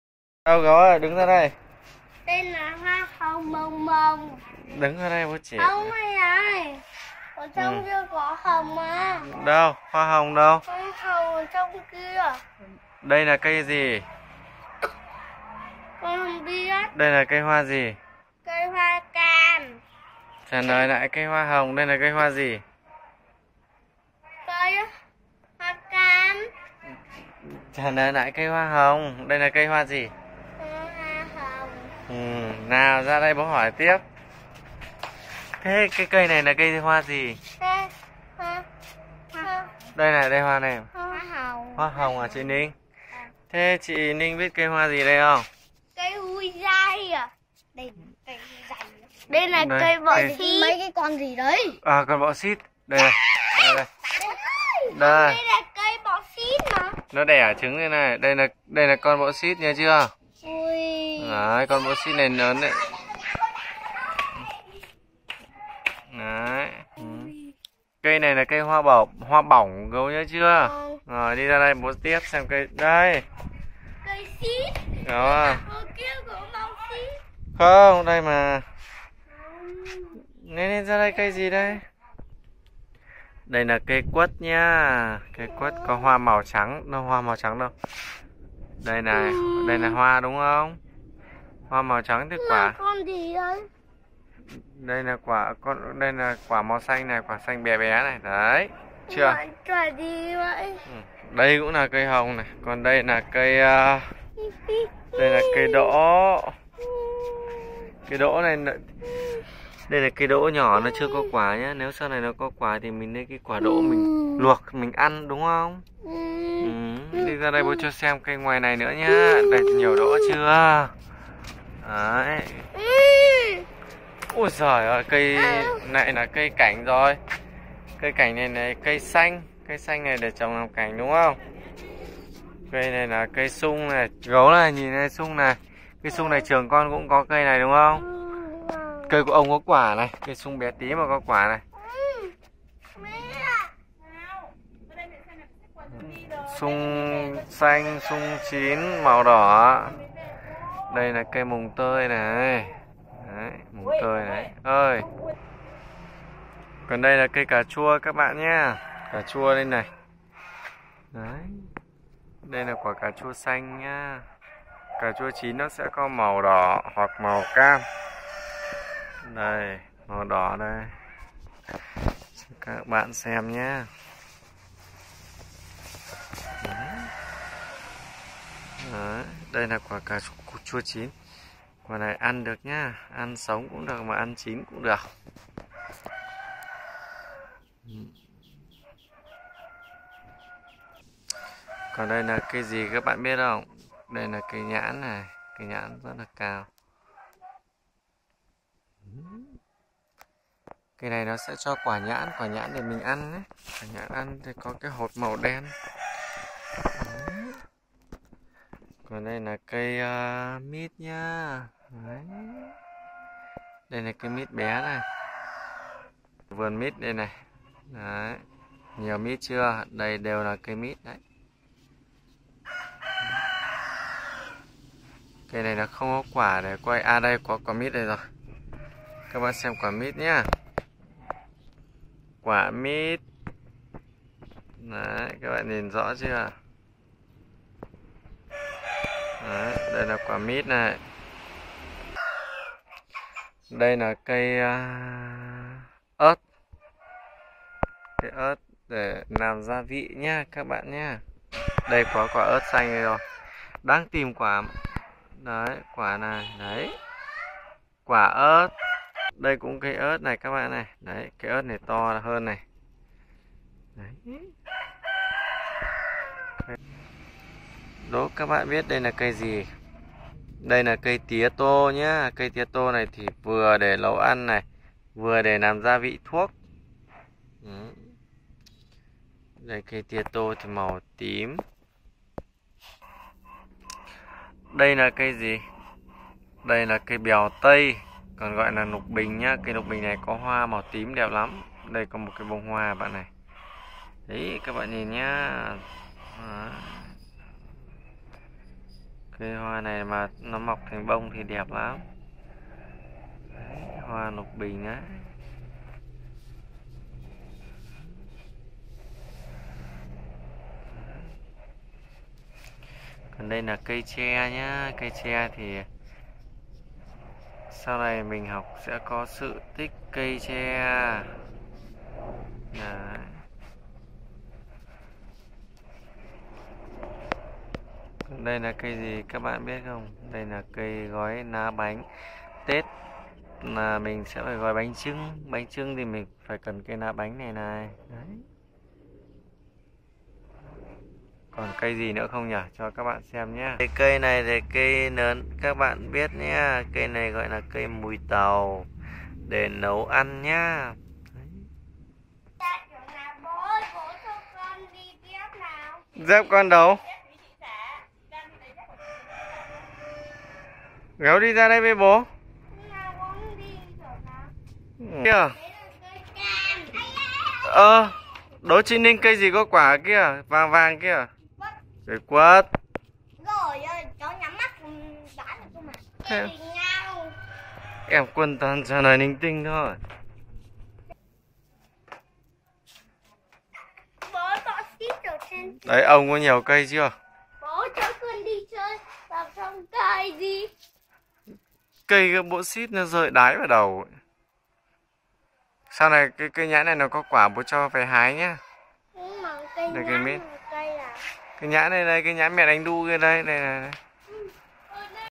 Dạy trẻ học cái loài cây ngoài thực tế | Học cây quanh nhà | MB Channel
Gấu về ông bà nội, được bố cho học các cây quanh nhà. Mọi người học cùng nhé